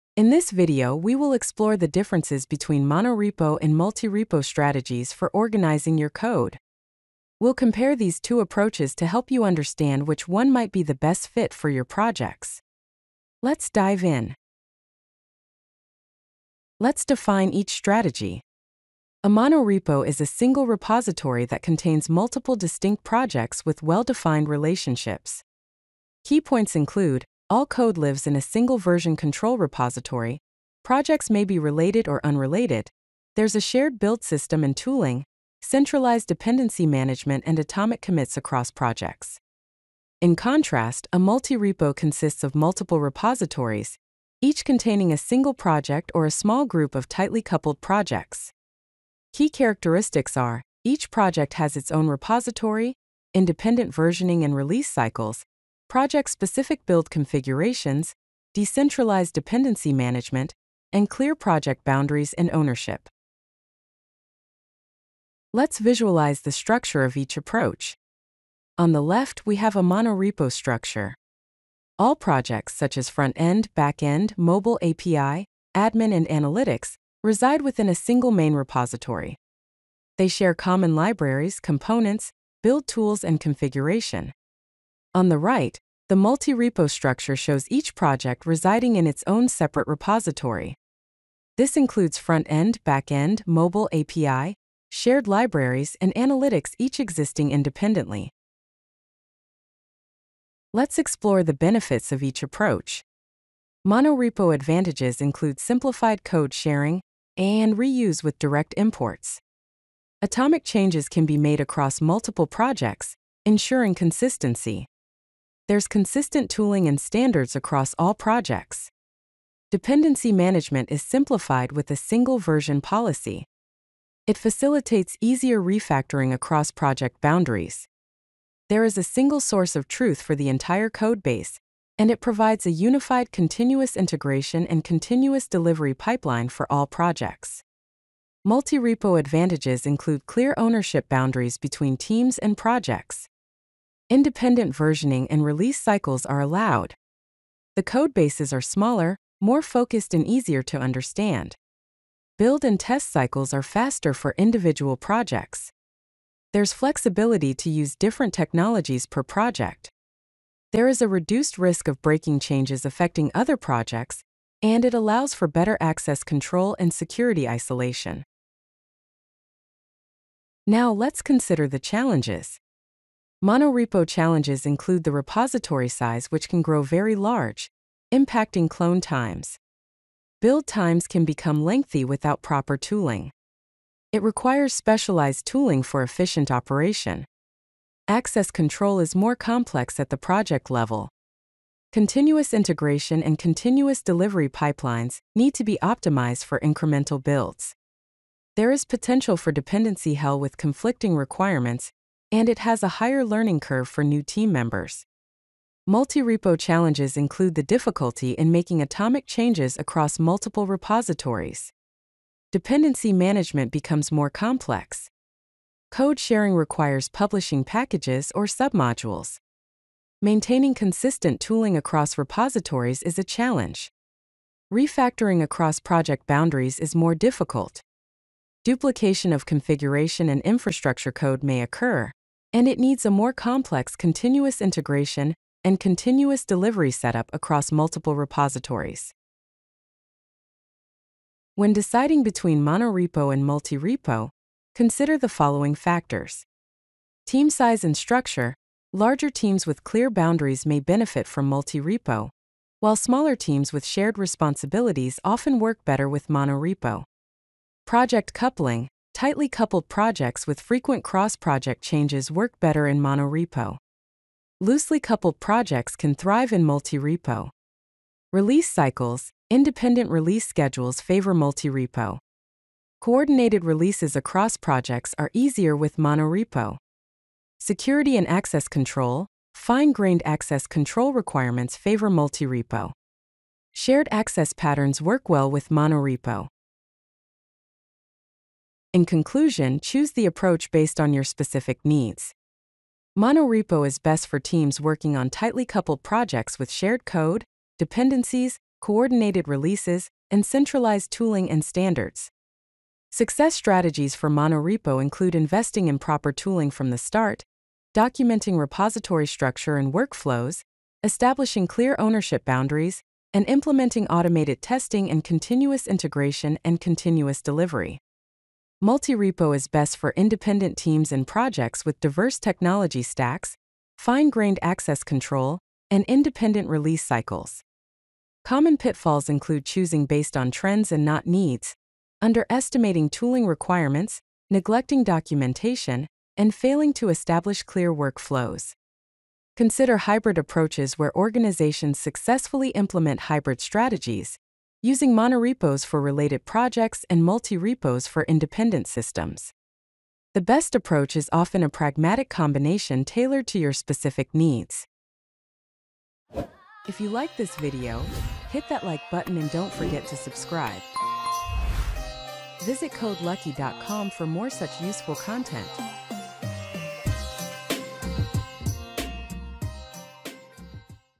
Monorepo vs Multi-repo: Which Code Management Strategy is Right for You?
Confused about Monorepo and Multi-repo strategies for your projects? 🤔 You're not alone! This video breaks down the key differences between these two popular code management approaches, helping you choose the best fit for your team and projects.

We'll explore:
* 📚 What Monorepos and Multi-repos are, with clear definitions.
* 🏗️ A side-by-side comparison of their structures, so you can visualize how they work.
* ✅ The advantages of each approach, from code sharing in Monorepos to independent releases in Multi-repos.
* ❌ The challenges to watch out for, like large repo sizes in Monorepos and dependency management in Multi-repos.
* 💡 Key factors to consider when making your decision, including team size, project coupling, and release cycles.
* 🏆 Best practices for success with either strategy.
* 💫 Hybrid approaches and why they might be the perfect balance.

Whether you're a seasoned developer or just starting out, this guide will equip you with the knowledge to make an informed decision about your repository organization. Let's optimize your workflow! 🚀

Chapters:
00:00 - Monorepo vs. Multi-repo
00:15 - What Are Monorepo & Multi-repo?
01:04 - Repository Structure Comparison
01:39 - Advantages of Each Approach
02:49 - Challenges of Each Approach
04:00 - Making the Decision: Key Factors
04:47 - Conclusion & Best Practices
05:56 - Outro